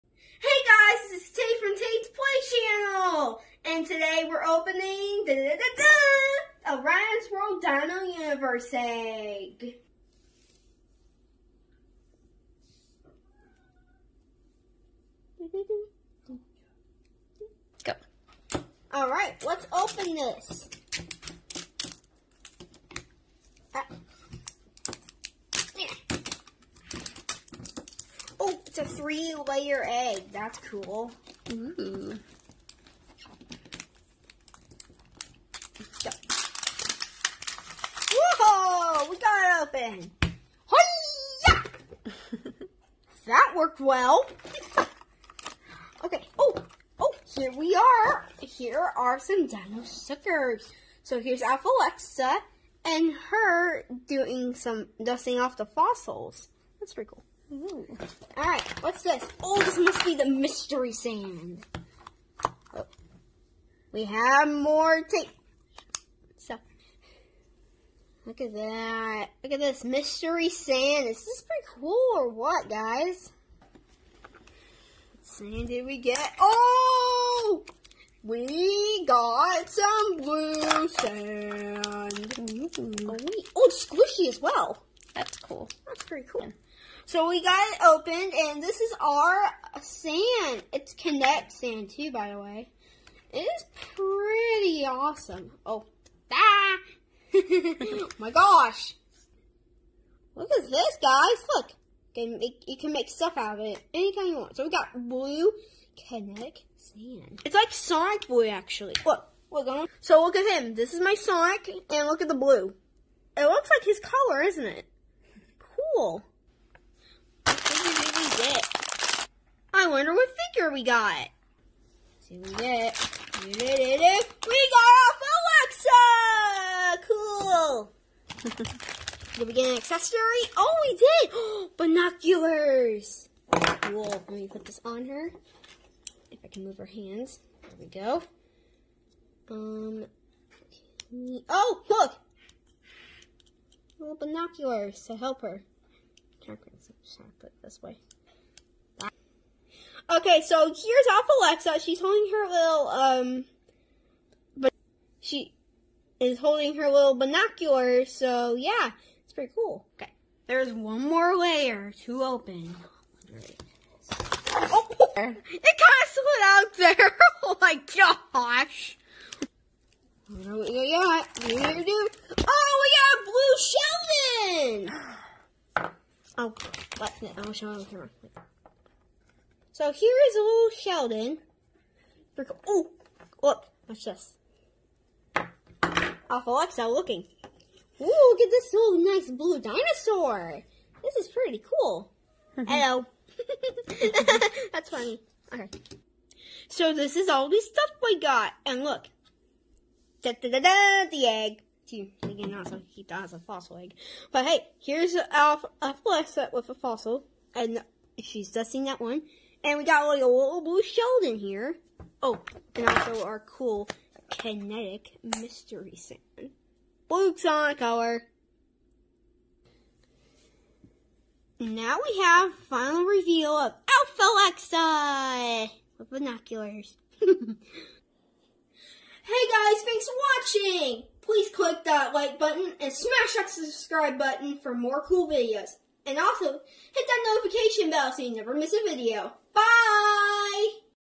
Tate opens the Ryan's Dinosaur Universe Egg
Let's see what's inside this Ryan"s Dinosaur Universe Egg!